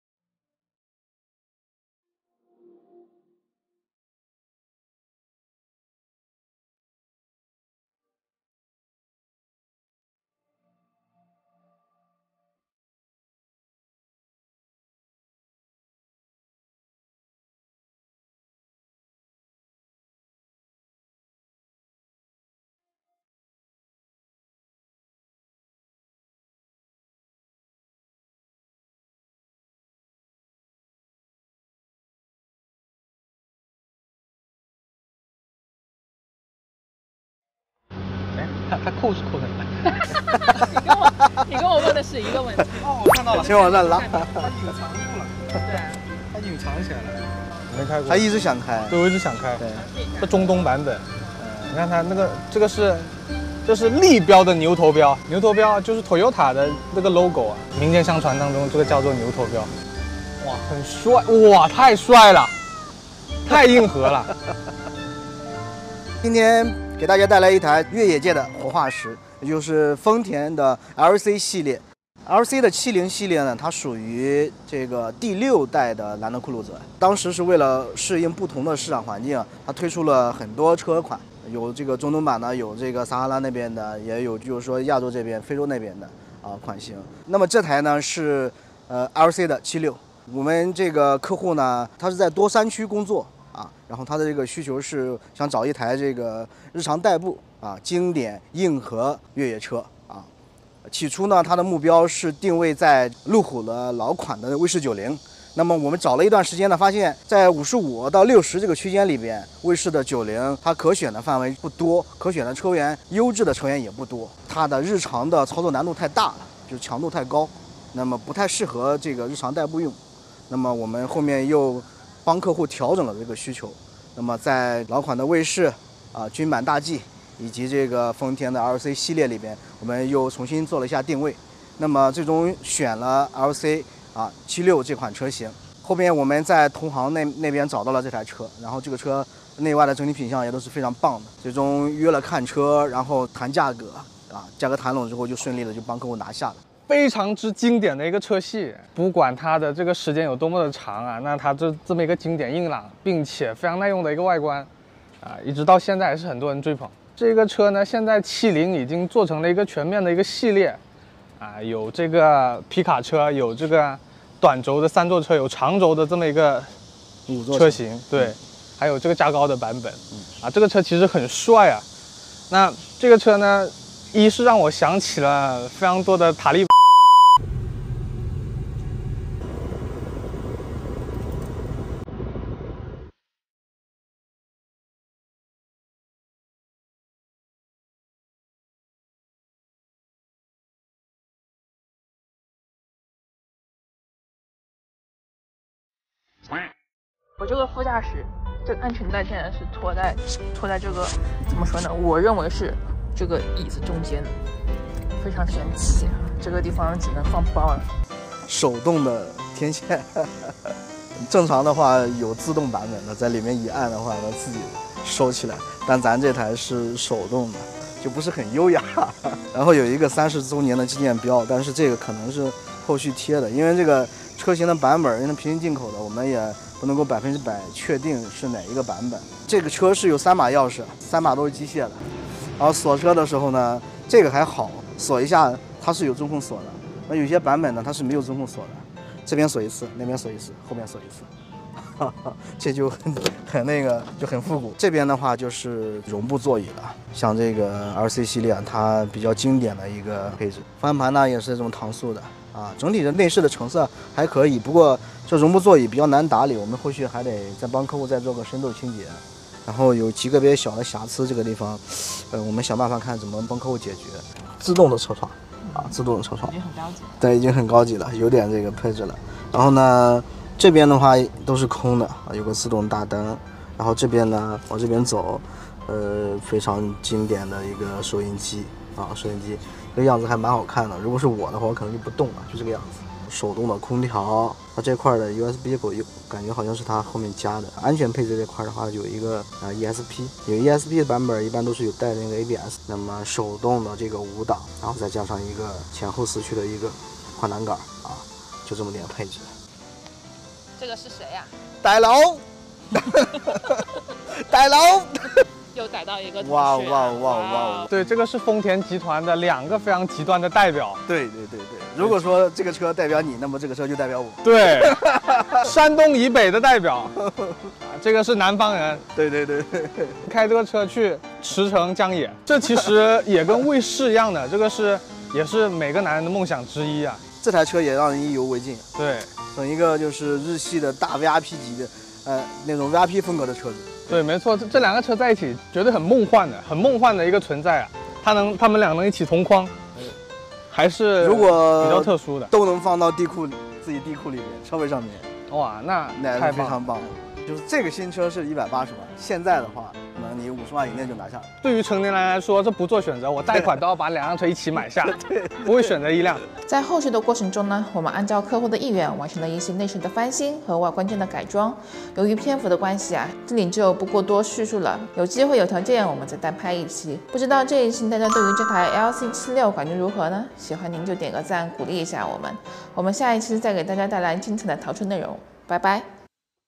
太硬了！越野车里的活化石——二手丰田酷路泽Cruiser LC76
_________________★独家授权★_________________
太硬了！越野车里的活化石——二手丰田酷路泽LAND CRUISER LC76
★购买可靠认证二手车请认准 “爱车的诺诺”唯一官方微信公众号
★实事求是的二手车★